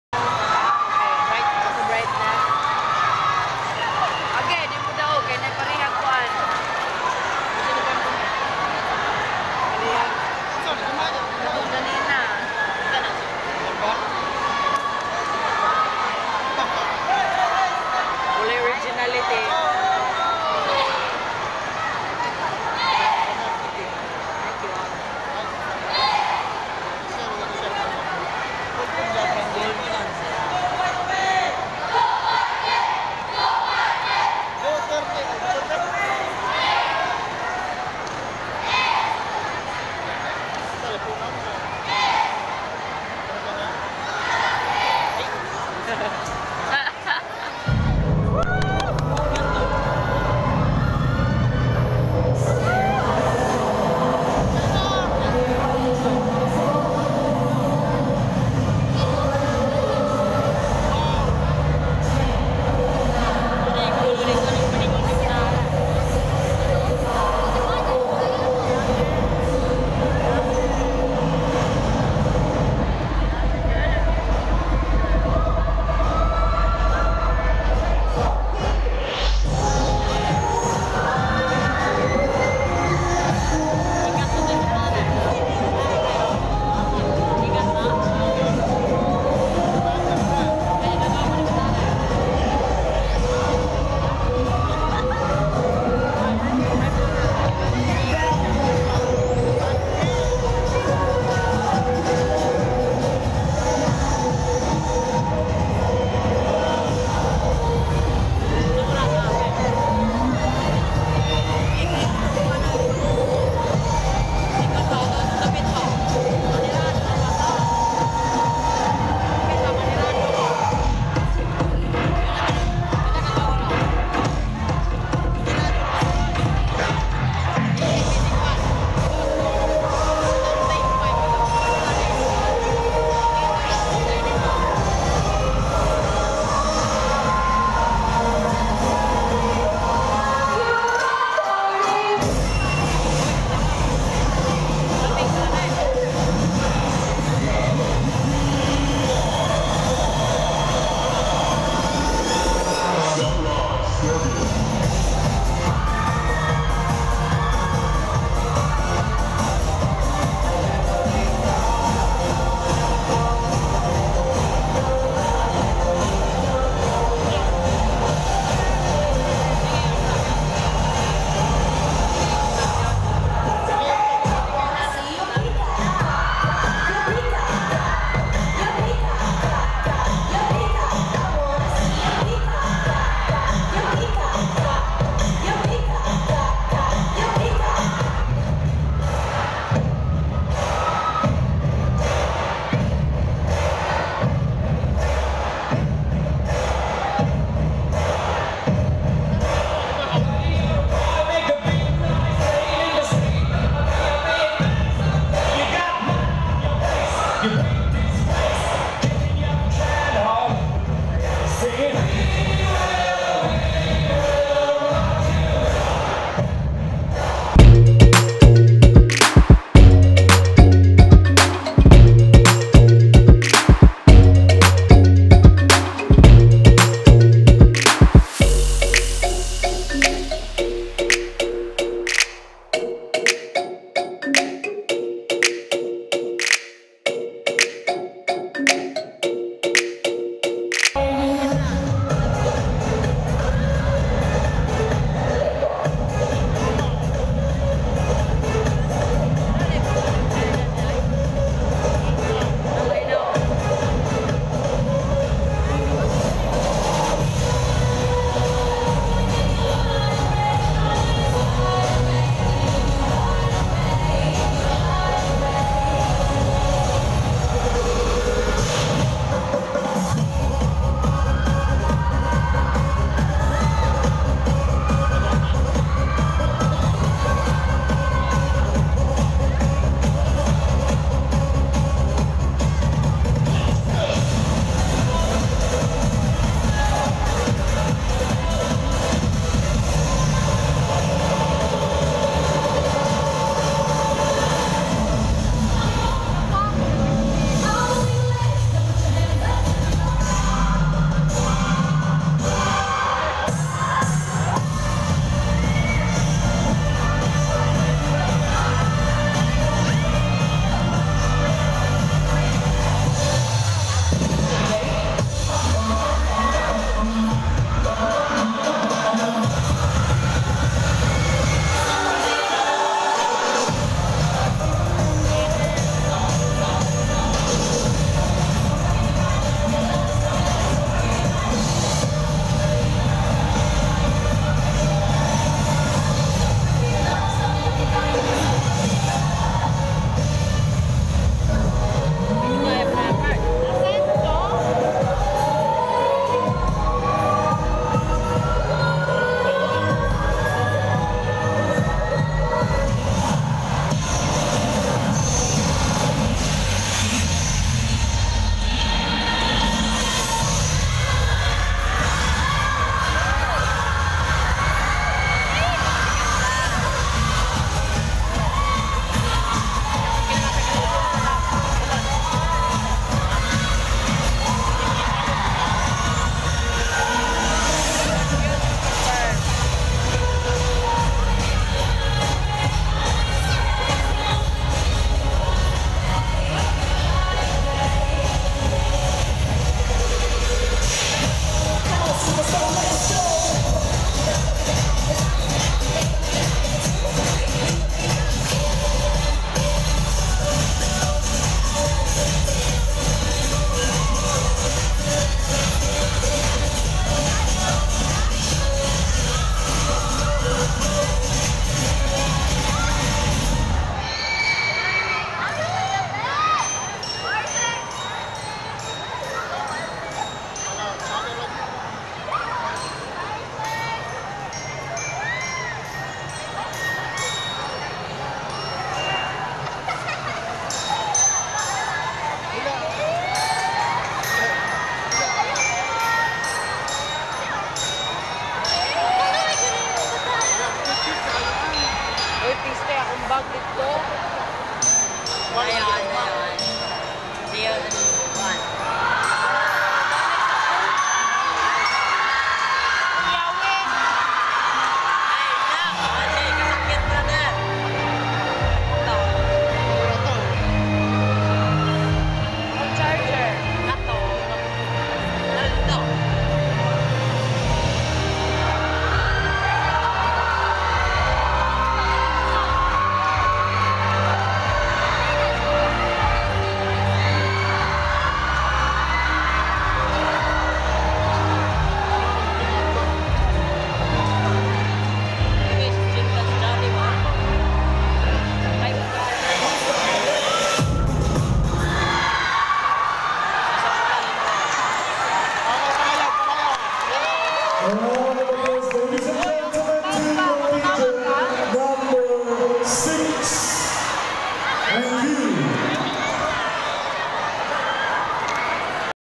Punong National High School Cheerdance Competition | Araw ng Bayan ng Siquijor 2019
Hello Everyone, I'm Richie Ann Mag-usara, an aspiring dancer and youtuber from the Mystic Island of SIQUIJOR!. 💃♥️👸